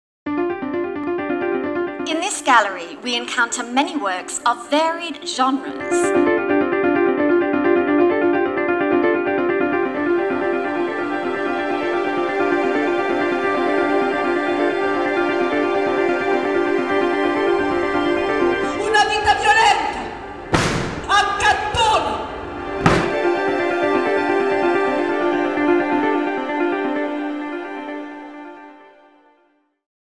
Coronet International Festival | Performance
Print Room at the Coronet presents an ambitious five-week festival of art from across the globe, spanning genres and defying expectations. Over 35 days the venue will host theatre, art, circus, dance and poetry with over 50 artists from 12 countries, celebrating great work, without borders, without agenda and without restriction.